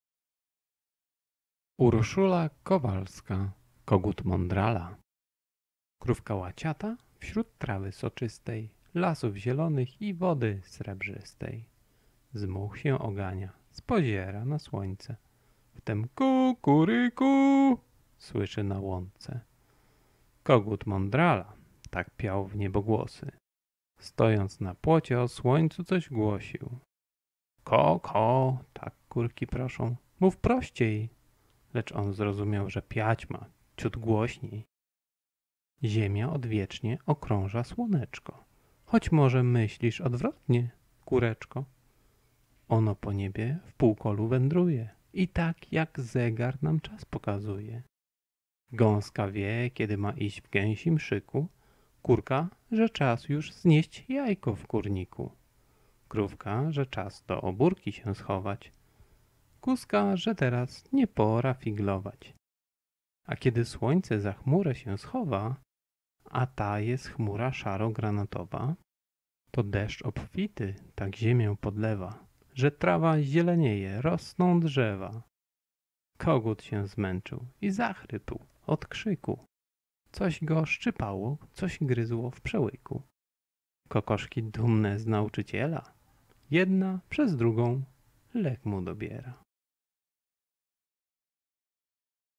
Urszula Kowalska - Kogut mądrala - wiersz - bajka
Urszula Kowalska - Kogut mądrala

Krówka łaciata wśród trawy soczystej,
lasów zielonych i wody srebrzystej,
z much się ogania, spoziera na słońce,
wtem „ku-ku-rykuuu!” słyszy na łące.

Kogut mądrala tak piał wniebogłosy,
stojąc na płocie o słońcu coś głosił.
Ko-ko – tak kurki proszą – mów prościej.
Lecz on zrozumiał, że piać ma ciut głośniej.

– Ziemia odwiecznie okrąża słoneczko,
choć może myślisz odwrotnie, kureczko?
Ono po niebie w półkolu wędruje
i tak jak zegar nam czas pokazuje.

Gąska wie kiedy ma iść w gęsim szyku.
Kurka, że czas już znieść jajko w kurniku.
Krówka, że czas do obórki się schować.
Kózka, że teraz nie pora figlować.

A kiedy słońce za chmurę się schowa,
a jest ta chmura szaro-granatowa,
to deszcz obfity tak ziemię podlewa,
że trawa zielenieje, rosną drzewa!

Kogut się zmęczył i zachrypł od krzyku,
coś go szczypało, coś gryzło w przełyku.
Kokoszki dumne z nauczyciela,
jedna przez drugą, lek mu dobiera.